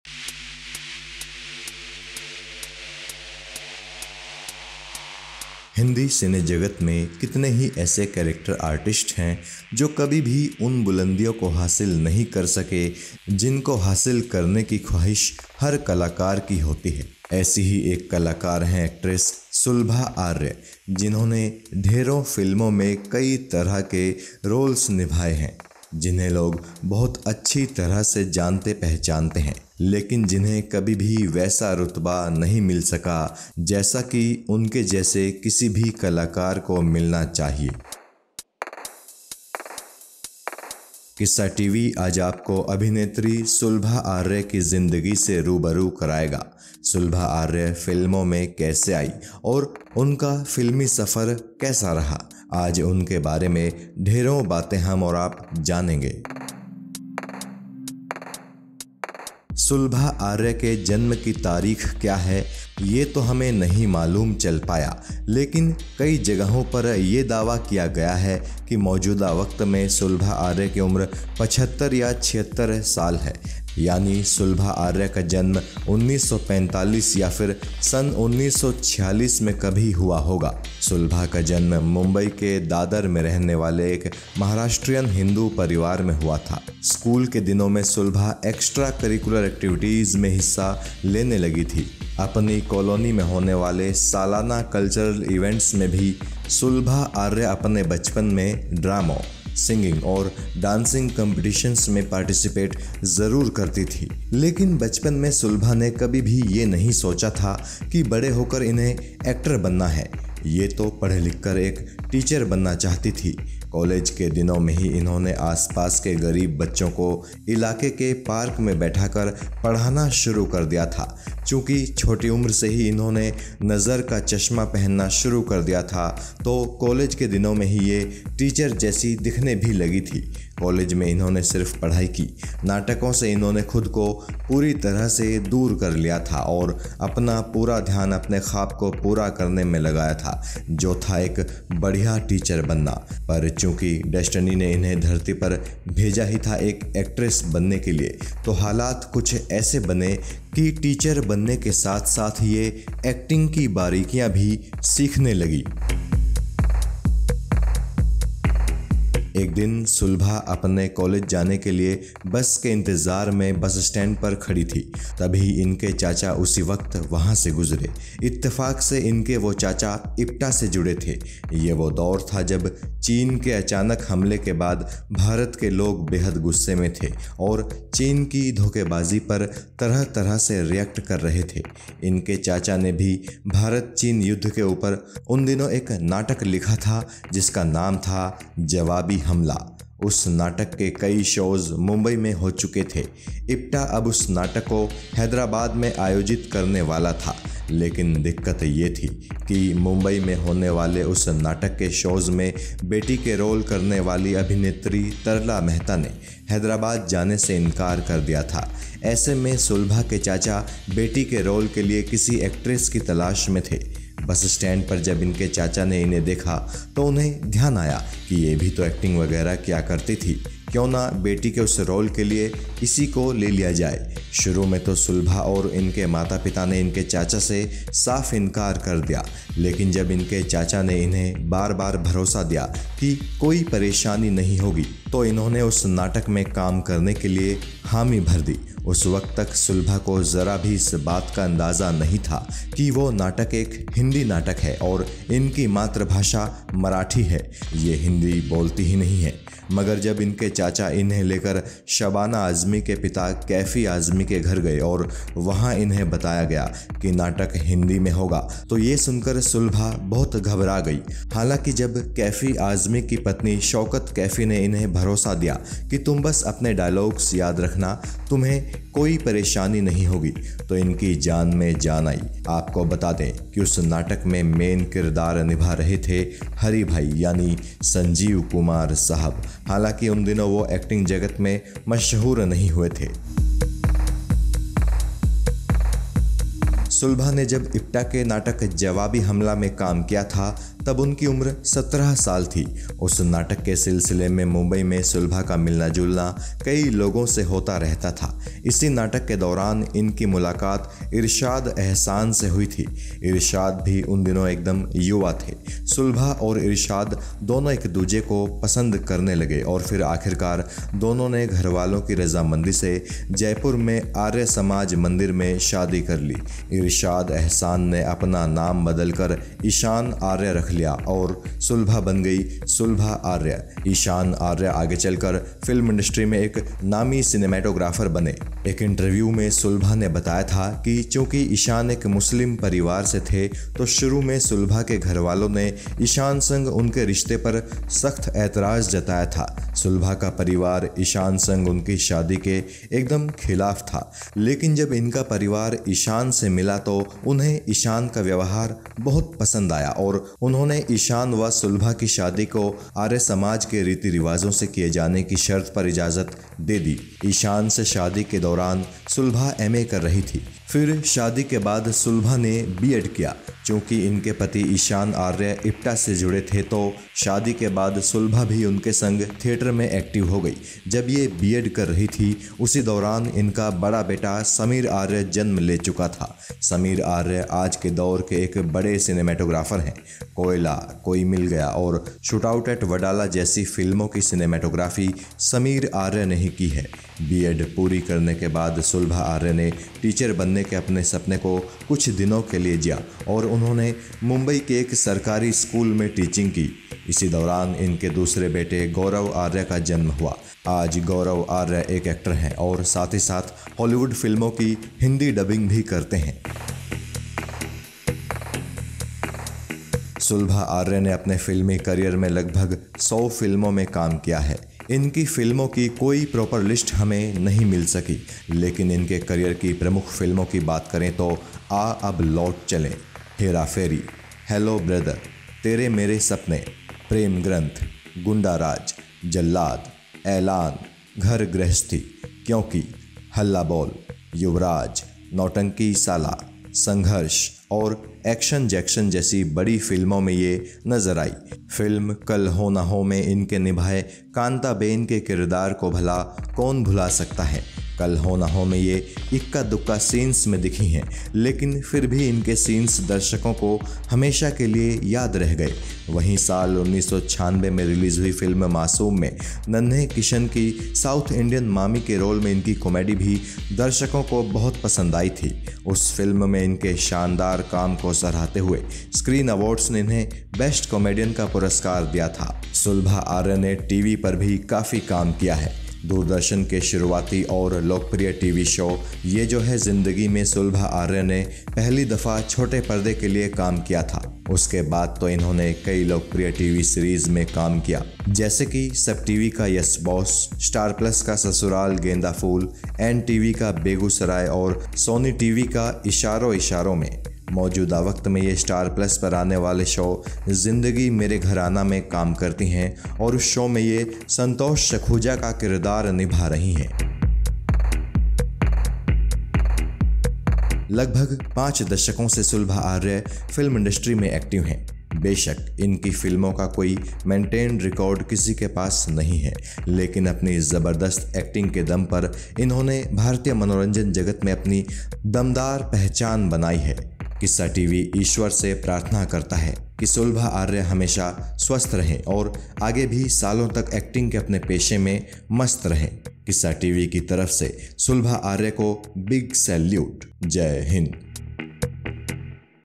Biography | Kal Ho Na Ho की Kantaben Actress Sulbha Arya की शानदार कहानी
Sulabha Arya is an Indian actress in Hindi and Marathi film, television and stage industry. Sulbha is another variation for the same name. She is the wife of late veteran Indian cinematographer Ishan Arya and mother of cinematographer Sameer Arya and actor Sagar Arya. She is best known for her role of Shanti Masi in Sasural Genda Phool. She also acted as Lakshmamma in Shyam Benegal's Amaravati ki Kathayein.

Arya was part of the Indian television industry's first sitcom, Yeh Jo Hai Zindagi, aired in 1984. She played the role of Kanta Ben in the 2003 dramedy film Kal Ho Naa Ho. She played the role of the mother in law in television series, Yes Boss on SAB TV. She was last seen at SET India's Ishaaron Ishaaron Mein as Geeta Shrivastav. She is a Maharashtrian who married Ishan Arya (Irshad Ahsan). Her son Sameer Arya is also a cinematographer, known for films like Koyla (1997), Koi... Mil Gaya (2003) and Shootout at Wadala (2013).